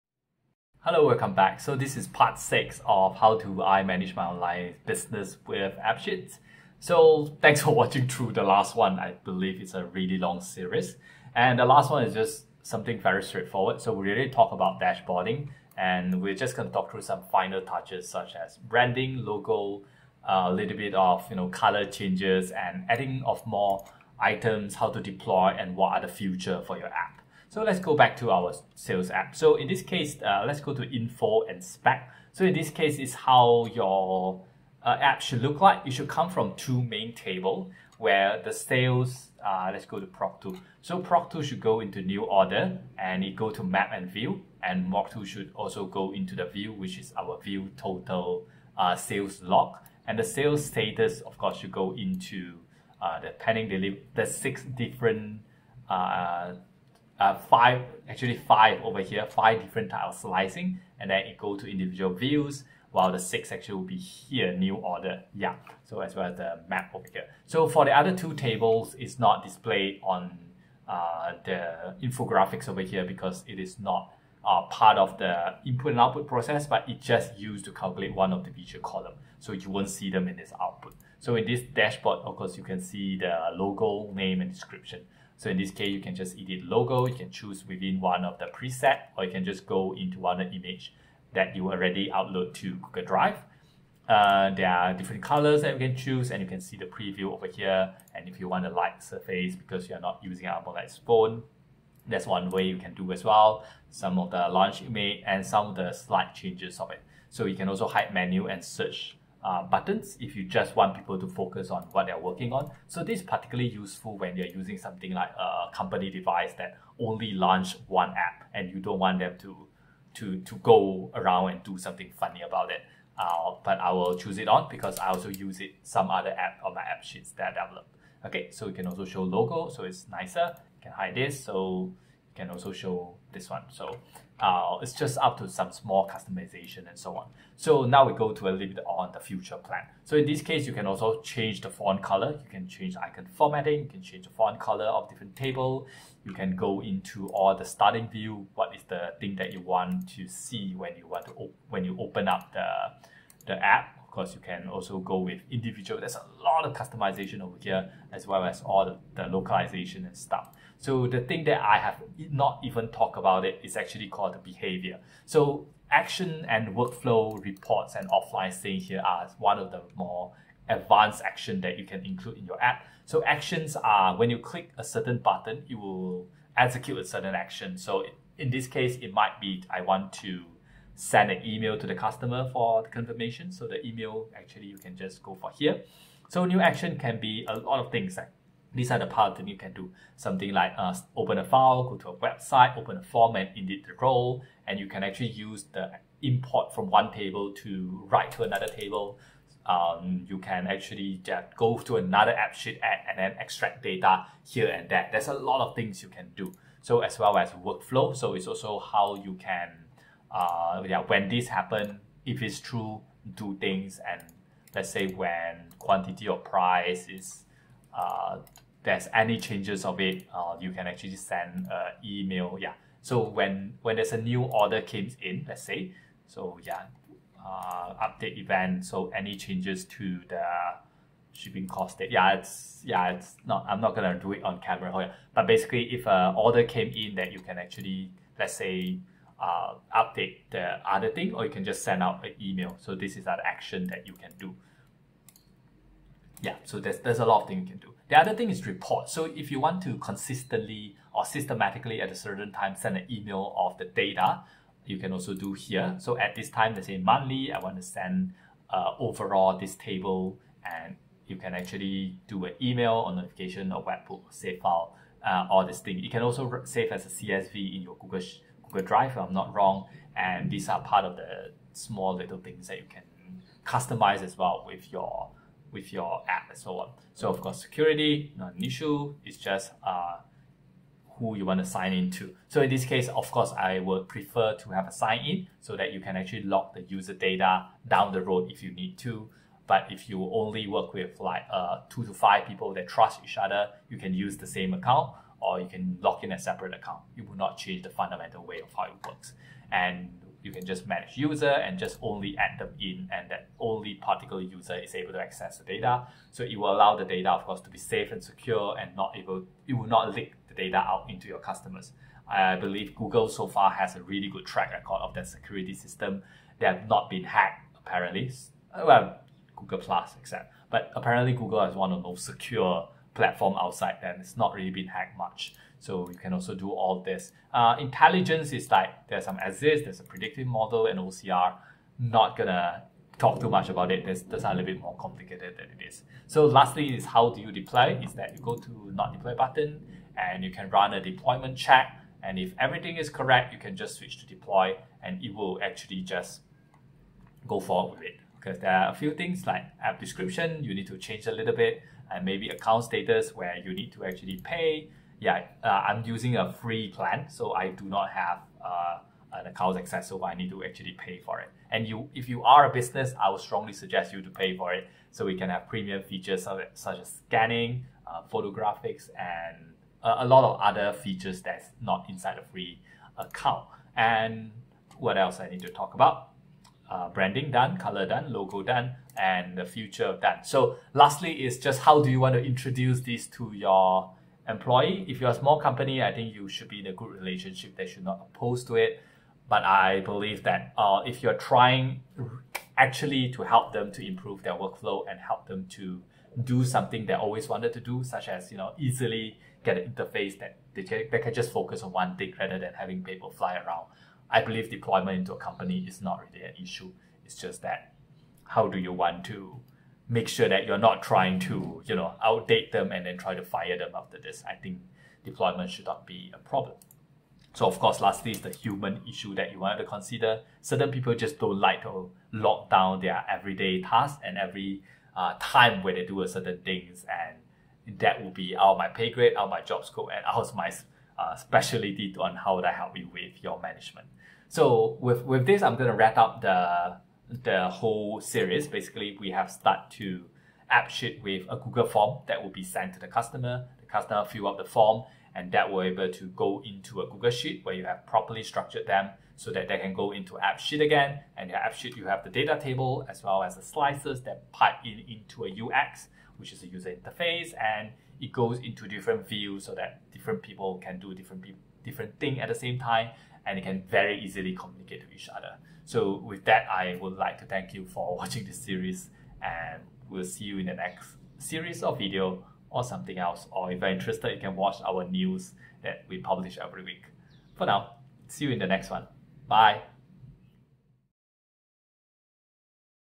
Online Business App for FREE | Appsheet Tutorial Part 6
If you are running your own small online business and need better communication between your staff members, Appsheet allows for a small number of users to use their platform for free.

Make your own business and logistic management app for free here in this six part series. Due to restriction on my free account, I wont be able to share the app directly in the public domain, but please contact me if you need a clone of the project.